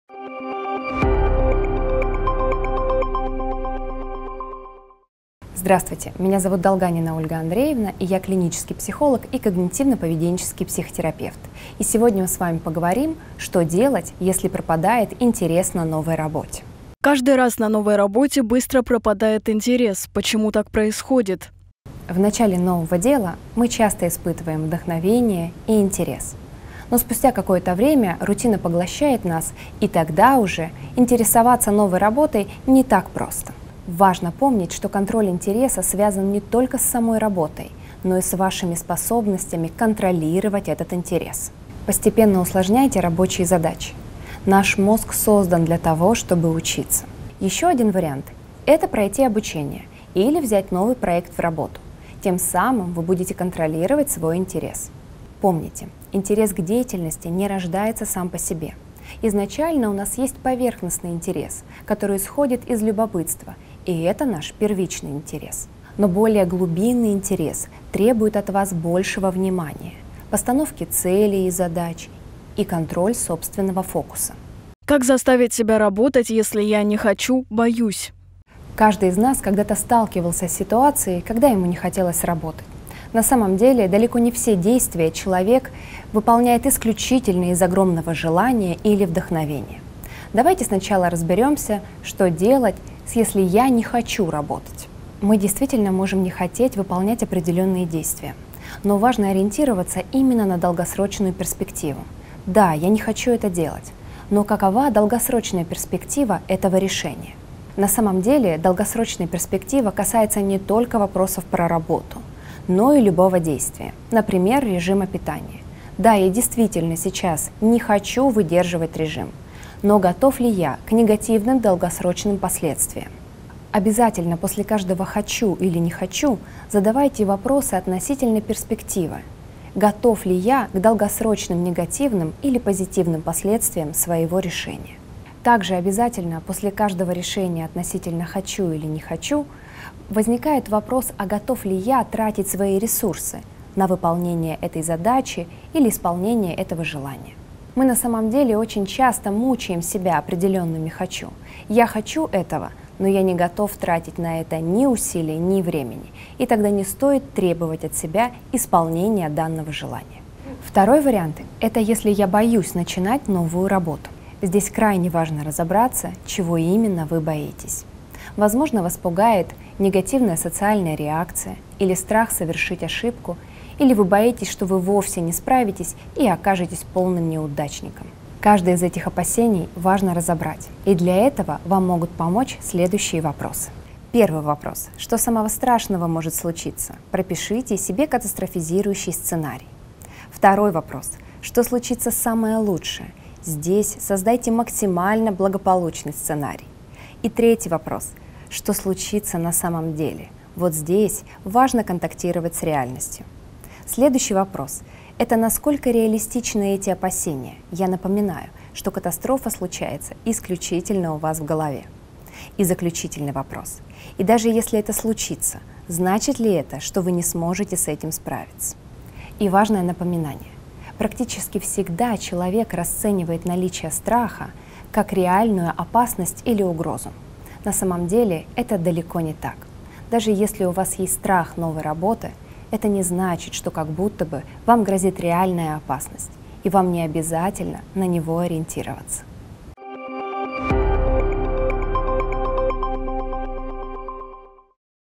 Что делать, если пропало желание работать? / «Вопросы психологу»
Эксперт Ольга Долганина рассказала, как повышать собственный интерес в профессии и что делать, если пропало желание работать? Ответы ищите в рубрике «Вопросы психологу».